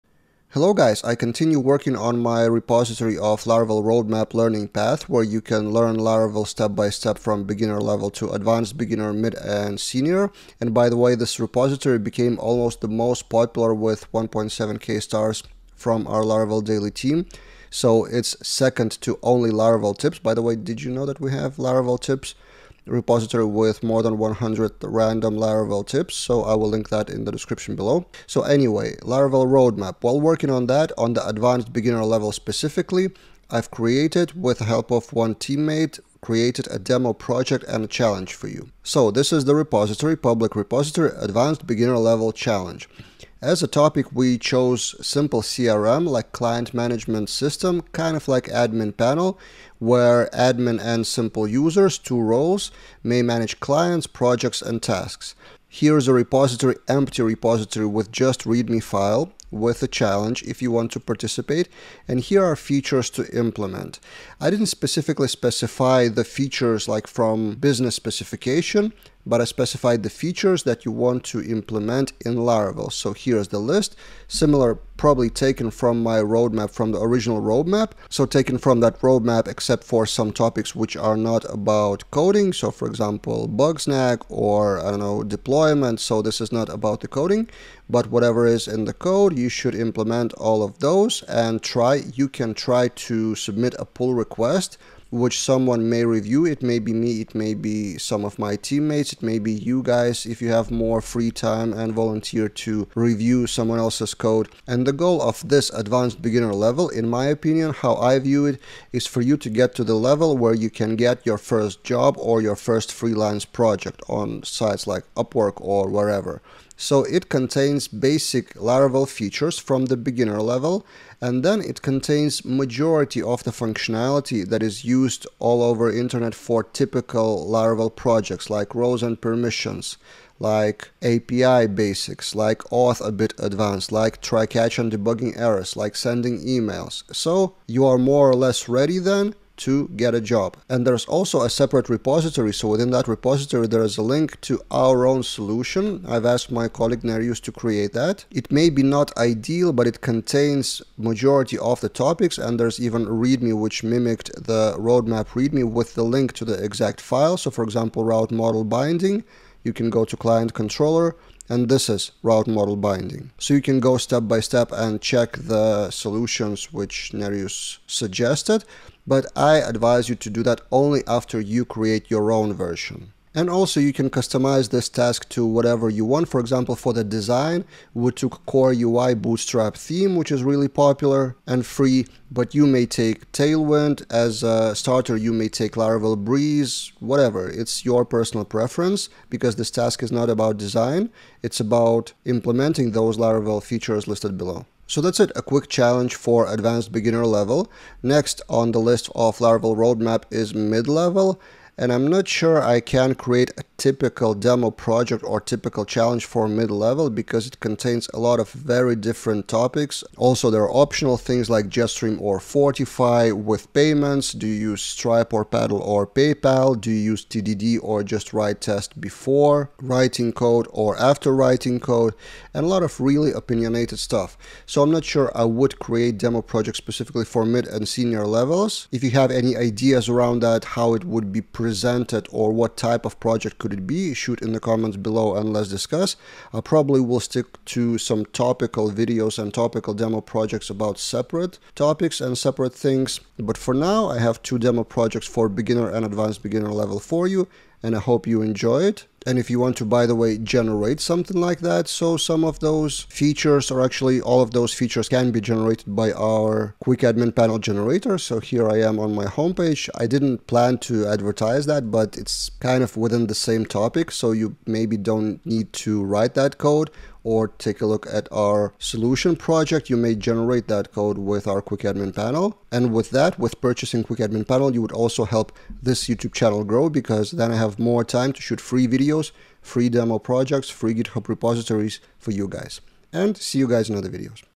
Laravel Advanced Beginner: Challenge + Demo Project
I continue working on my Laravel Roadmap Learning Path, and as a part of that, I released a challenge for the Advanced Beginner level. Let's discuss what's inside.